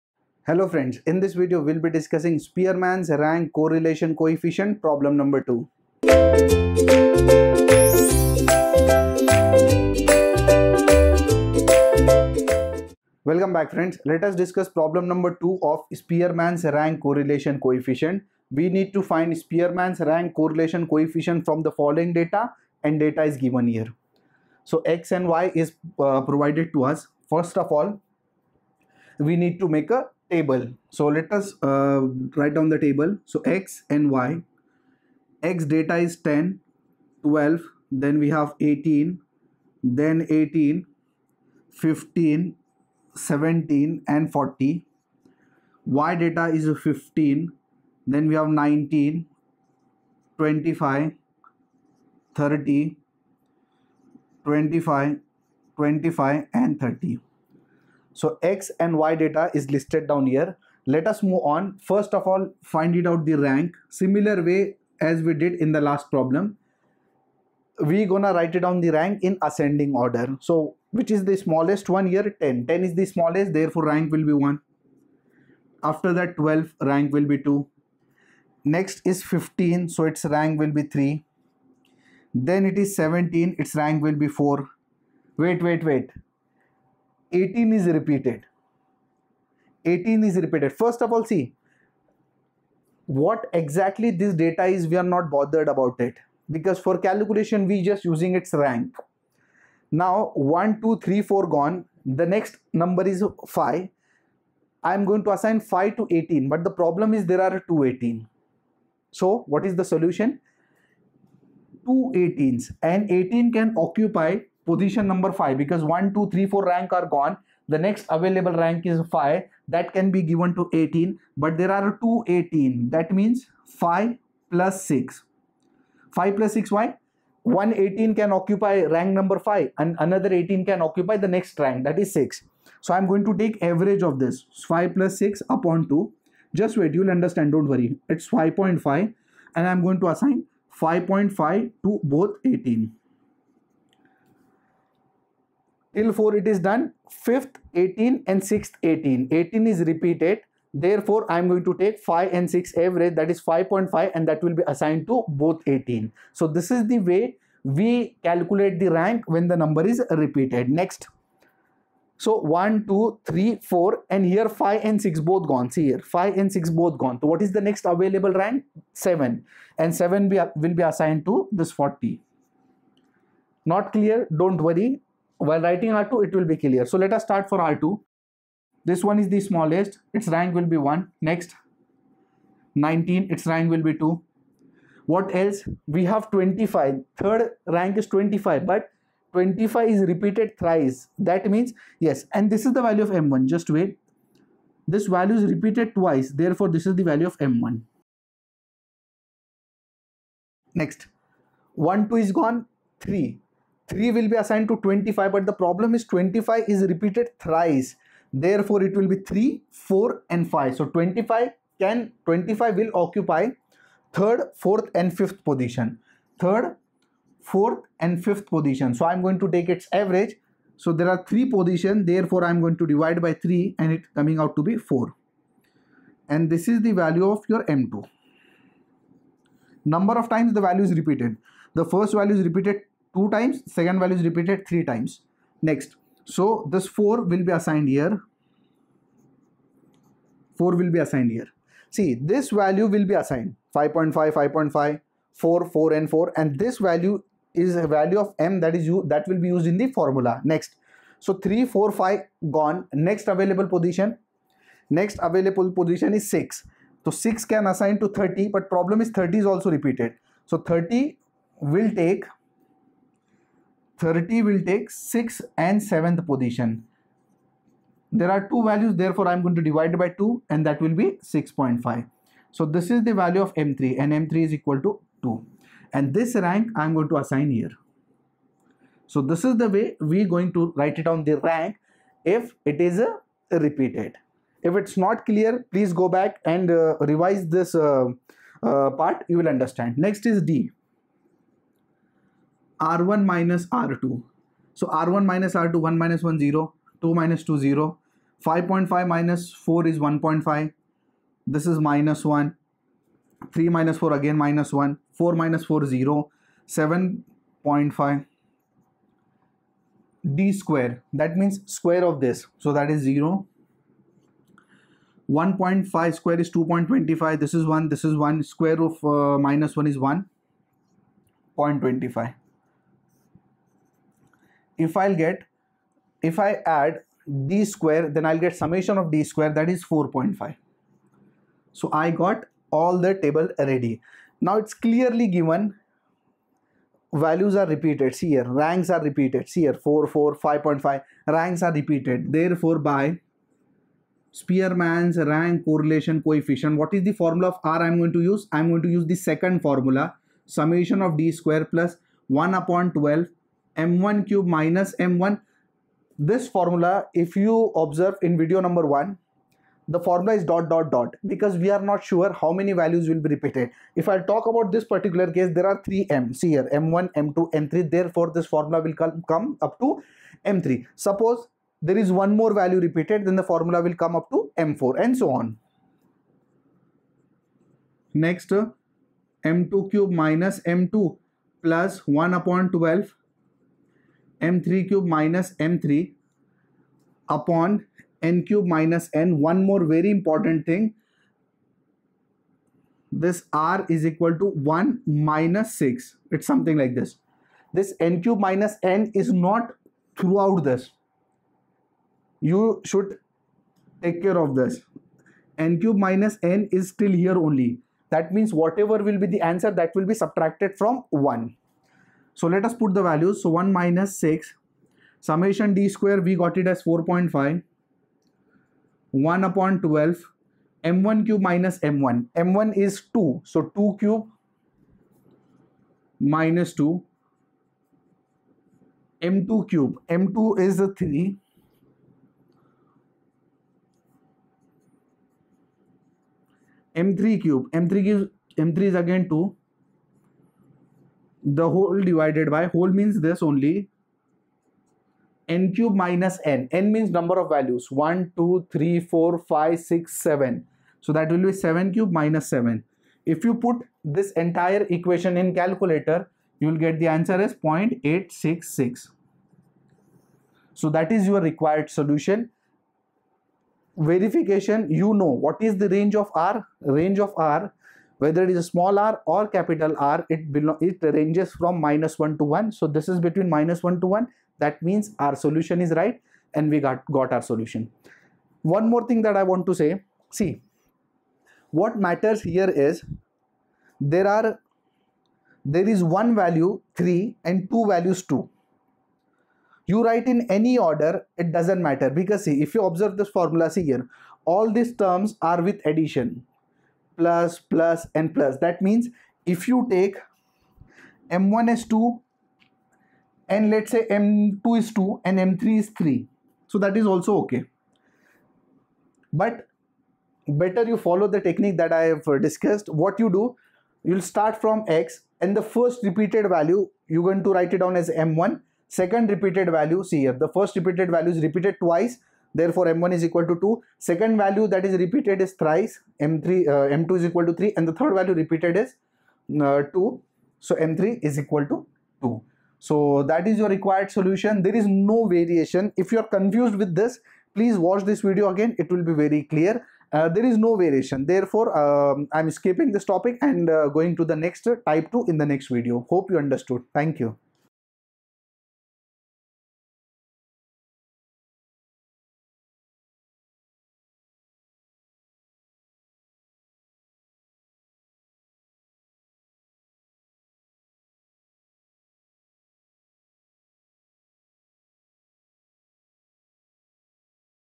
Spearman's Rank Correlation Coefficient - Problem 2 - Engineering Mathematics 3
Video Name - Spearman's Rank Correlation Coefficient - Problem 2

Chapter - Correlation and Regression

Happy Learning